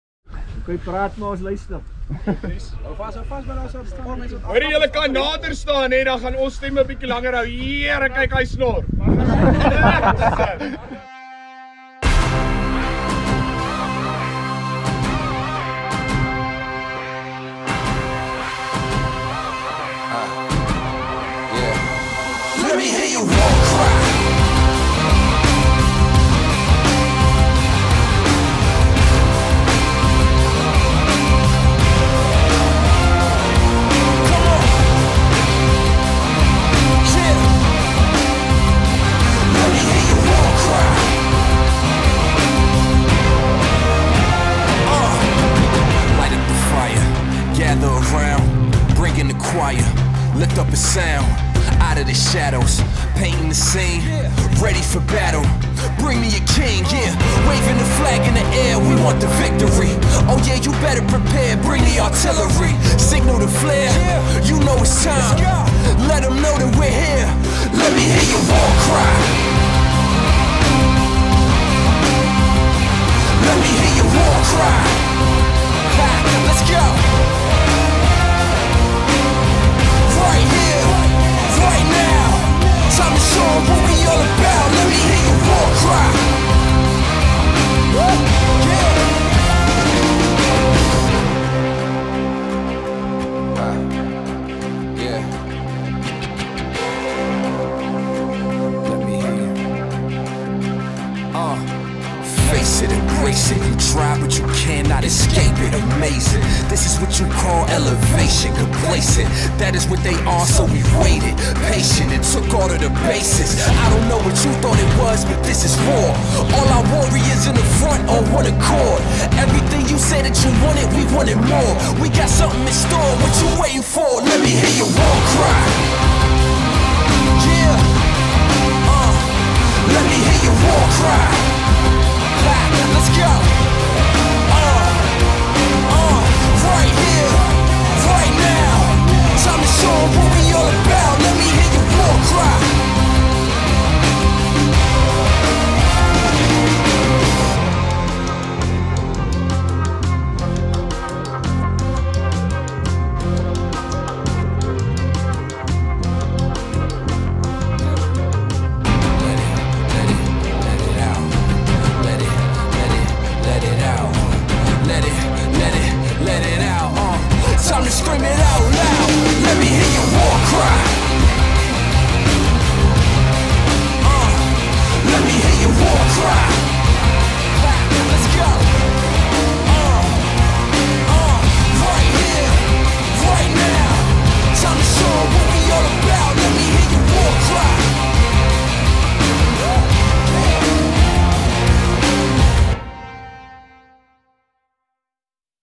RAW 4x4 Fun Day: Atlantis Dunes
We had a blast at the RAW 4x4 Fun day on the 23rd of April. The obstacles where manageable, even with a standard 4x4 and the prizes where out of this world.
If you couldn't make it you definitely missed out. It was a huge jol.

Thanx to Dries and Arno for taking care of us on the day. We got lost a number of times. Hahahah, but it was good fun. What a place to get lost in is Atlantis Dunes.